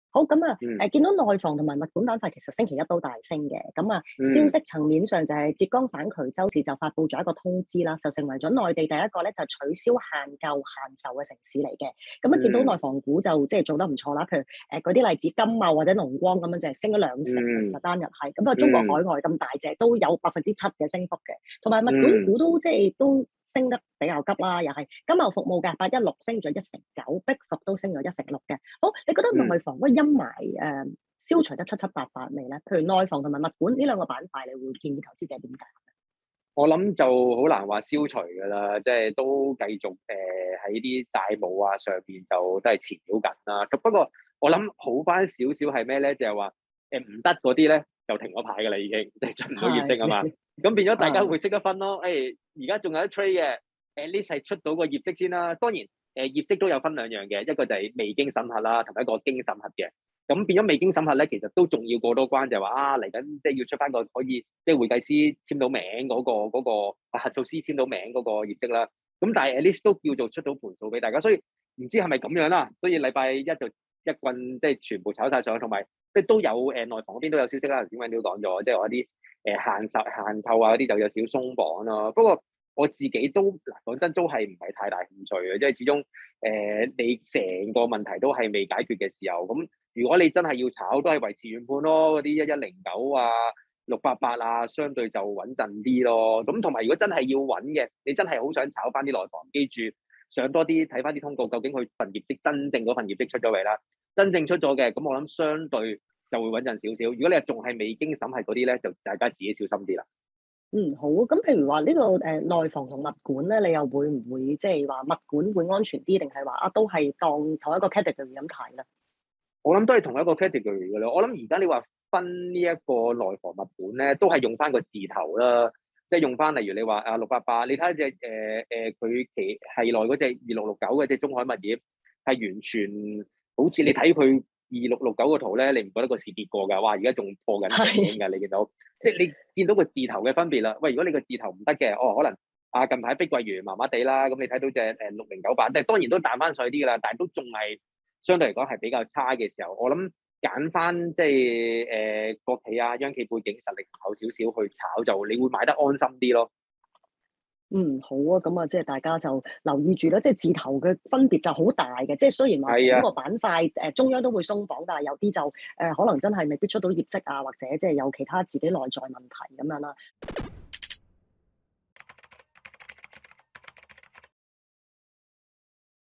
【ET開市直擊】（精華）內房股逆市續升追入貼士 唐牛揀2隻內房股、1隻物管股
內房股去年起受國策監管債務問題纏繞，近日有樓市漸鬆綁跡象，浙江省衢洲市首先取消限購及限售，福建省福州市也取消限購，重慶、山東菏澤、江西贛州、河南鄭州也調整限貸及降首付措施。

受消息刺激，本周一（4日）內房及物管股大升，兩隻國企內房股中海外（00688）、潤地（01109）分別升7.56%及3.49%，民企內房龍湖（00960）和碧桂園（02007）也造好升4.52%及8.36%，萬科（02202）、雅居樂（03383）、旭輝（00884）及龍光（03380）更急升10%至20%。物管股方面，金茂服務（00816）升19%、碧桂園服務（06098）升16%。

獨立股評人唐牛分析內房及物管板塊指，債務陰霾難於短期內消除，無法公布經審核業績的內房相繼停牌，想買此板塊的投資者，必須查看清楚已公布業績的相關股份，是否已經過審核方夠安全。唐牛推介內房股可揀國企、央企字頭的潤地（01109）及中海外（00688），後者同系的中海物業（02669）亦跟字頭強勢破頂，回調買入會較安心。

**重溫【ET開市直擊】特首選舉對港股有冇影響  內地抗疫股炒上有得追？**